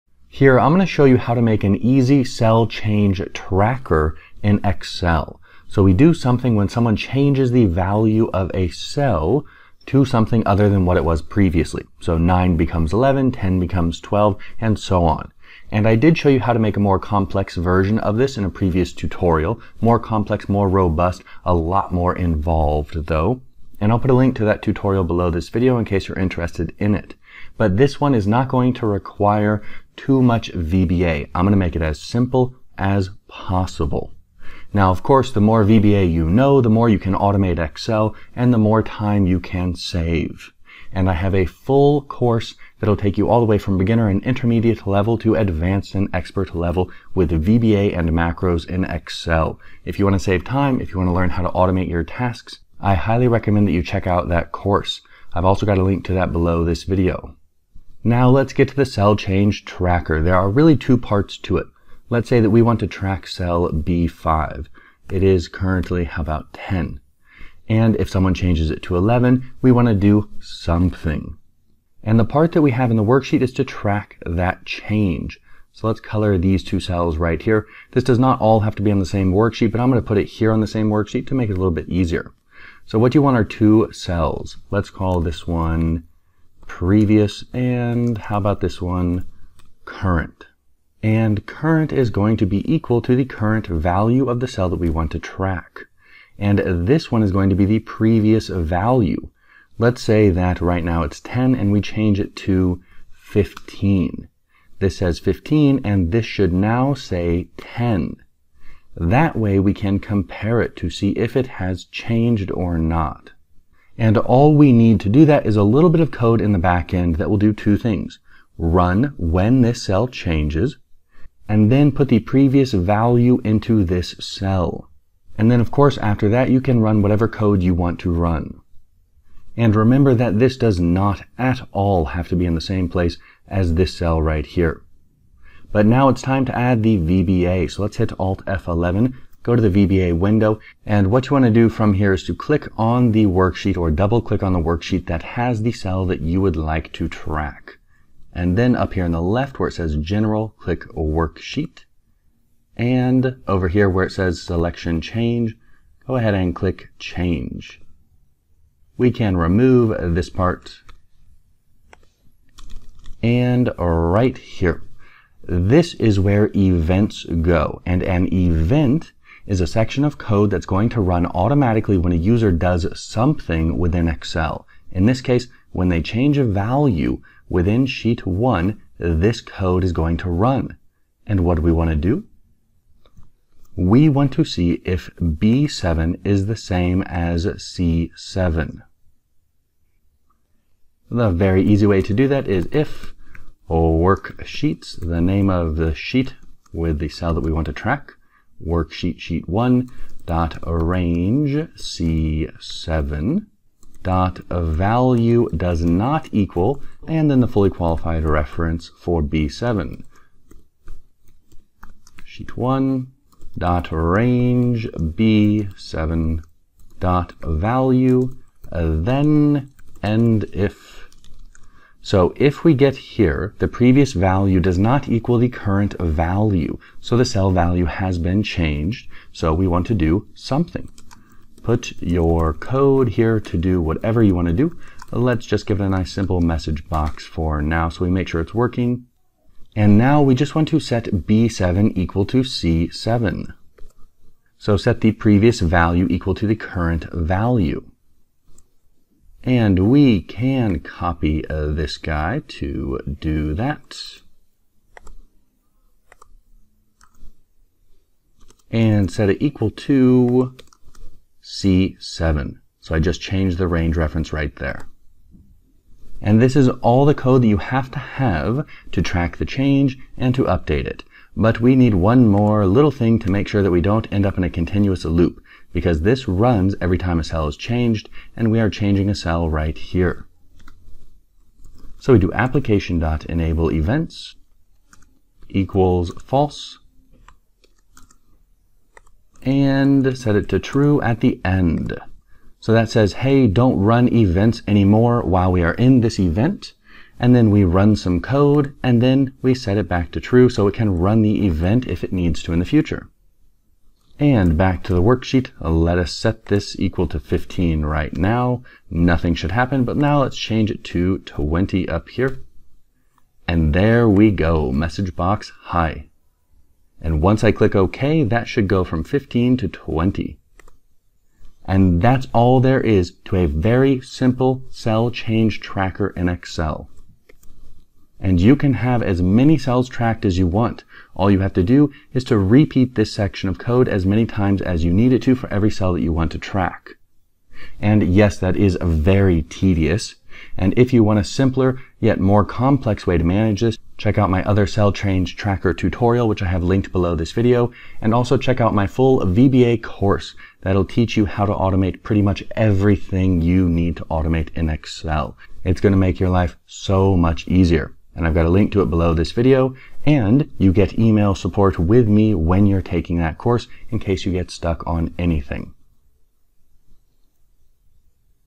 Easy Cell Change Tracker in Excel
The easy way to track cell changes in Excel and then do something when those cells are changed.

This method shows you a simple system that has 2 parts: one in the worksheet and one in VBA - but don't worry because I show you how to do everything and change it for your situation.

There is NO complex VBA in this tutorial, I promise! :)

To build powerful VBA projects or further customize this one, checkout my full VBA Course